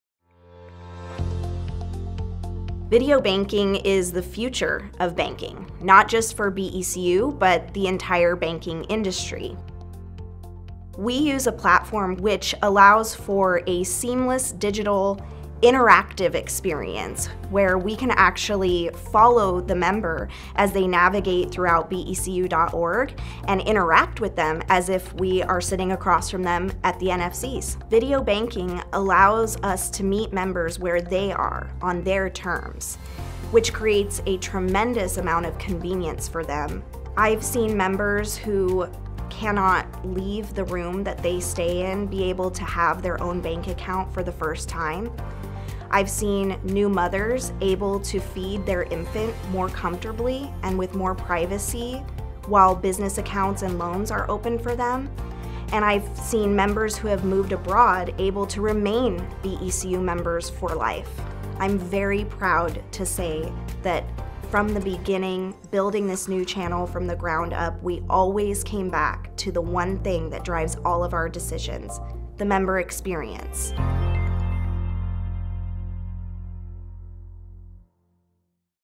Video Banking - 'The Future of Banking' | 2023 Member Summit | BECU
BECU members can fulfill financial needs from the comfort of home or any location.

We can assist you virtually with the following services.

- New accounts (including Early Saver accounts)
- Loans.
- Mortgage inquiries.
- Account inquiries.